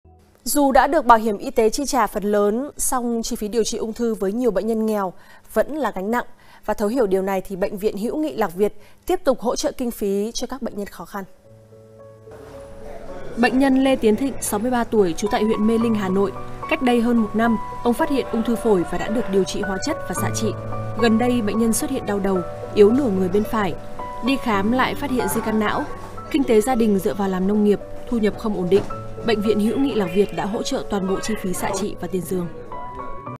Vì bệnh nhân ung thư 12/05/2024: Bệnh nhân Lê Tiến Thịnh
Dù đã được BHYT chi trả phần lớn, xong chi phí điều trị ung thư với nhiều bệnh nhân nghèo vẫn là gánh nặng. Thấu hiểu điều này, Bệnh viện hữu nghị Lạc Việt tiếp tục hỗ trợ kinh phí cho những bệnh nhân khó khăn.

Bệnh nhân Lê Tiến Thịnh 63 tuổi trú tại huyện Mê Linh, Hà Nội. Cách đây hơn 1 năm, ông phát hiện ung thư phổi, đã được điều trị hóa chất và xạ trị. Gần đây bệnh nhân xuất hiện đau đầu, yếu nửa người bên phải, đi khám lại phát hiện di căn não. Kinh tế gia đình dựa vào làm nông nghiệp, thu nhập không ổn định.

Bệnh viện hữu nghị Lạc Việt đã hỗ trợ toàn bộ chi phí xạ trị và tiền giường.
🏥 Bệnh viện Hữu Nghị Lạc Việt
📎 Nguyễn Tất Thành, Định Trung, Vĩnh Yên, Vĩnh Phúc

© Bản quyền thuộc về Bệnh viện Hữu Nghị Lạc Việt.